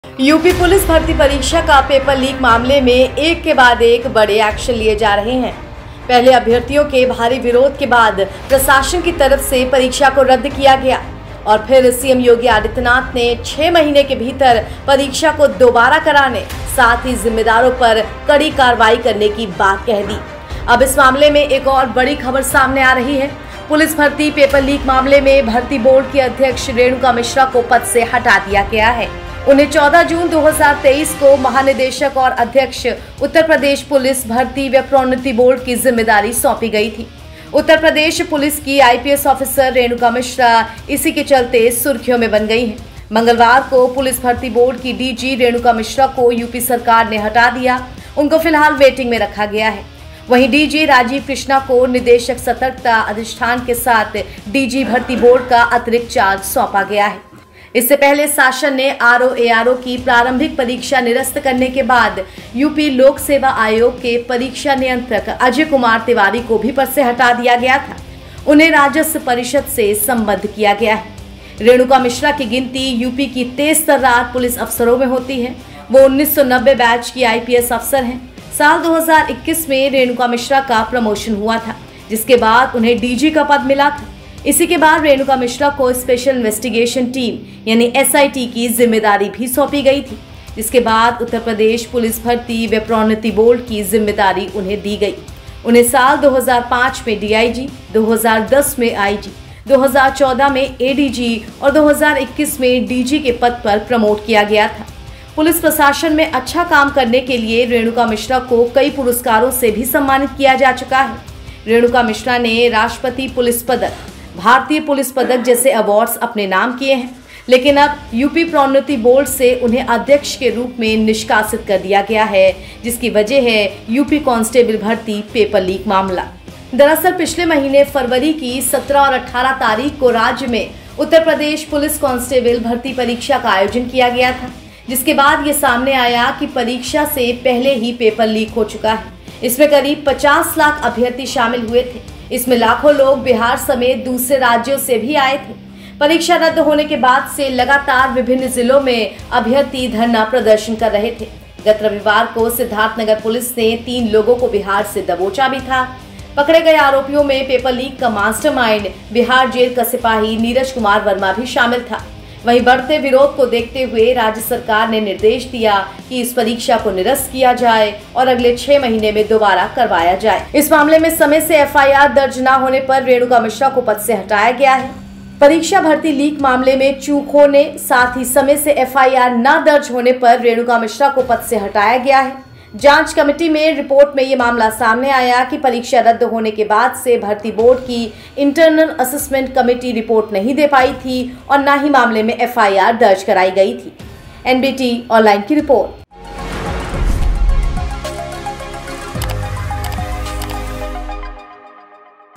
UP Police Paper Leak Case: IPS Renuka Mishra पर Action, Rajiv Krishna को अतिरिक्त कार्यभार
यूपी पुलिस भर्ती परीक्षा का पेपर लीक मामले में एक के बाद एक बड़े एक्शन लिए जा रहे हैं, पहले अभ्यर्थियों के भारी विरोध के बाद प्रशासन की तरफ से परीक्षा को रद्द किया गया, और फिर सीएम योगी आदित्यनाथ ने 6 महीने के अंदर परीक्षा को दोबारा करने, साथ ही जिम्मेदारों पर कड़ी कार्रवाई करने की बात कही गई।